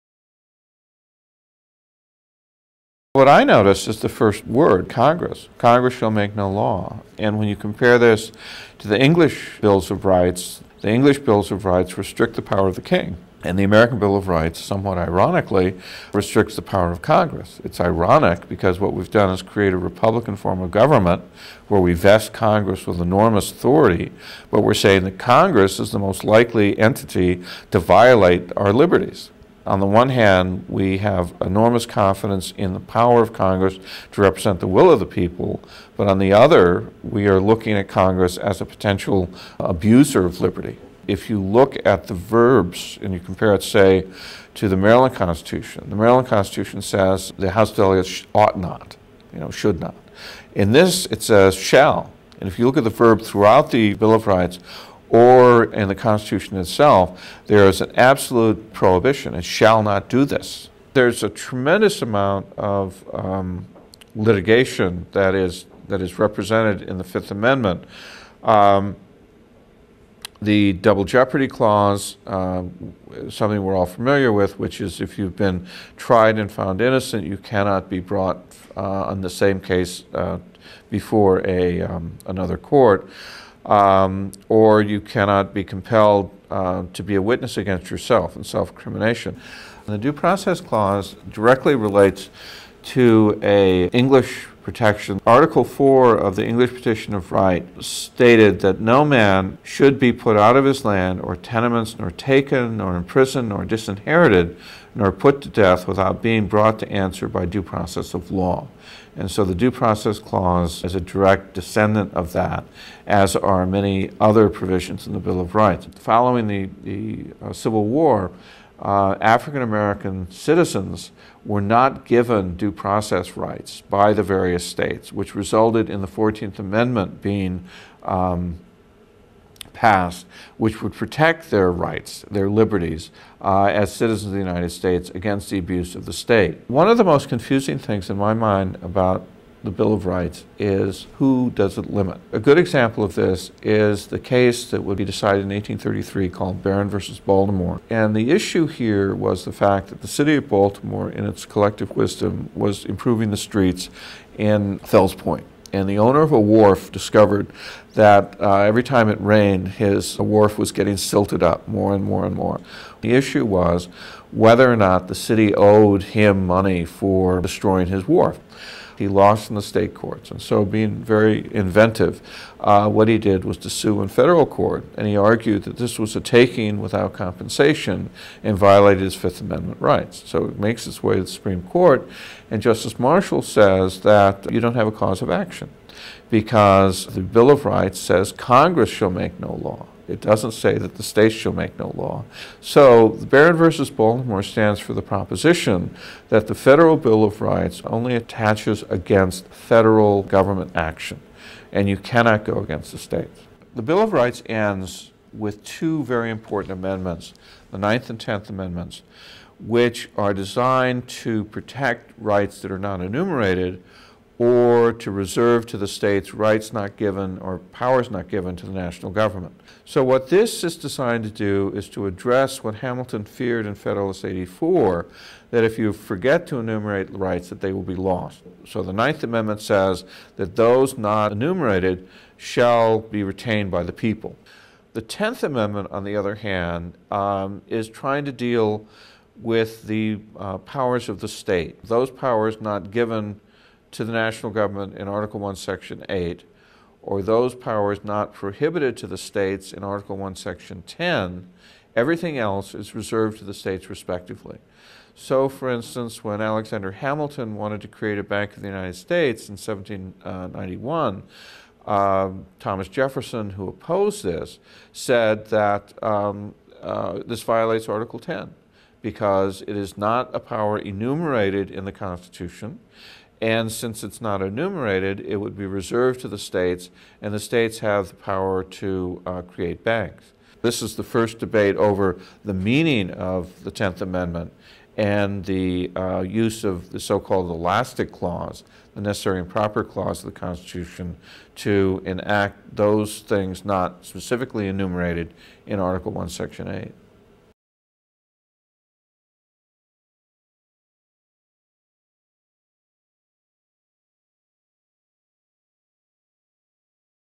Examples of Historical Thinking - Bill of Rights - Looking at the Amendments
Whitman Ridgway outlines some of the context in which the Bill of Rights was created and arguments surrounding its creation. He goes on to look at the specific language of several of the Amendments, the reasons for their wording and inclusion, and how he believes they should be interpreted.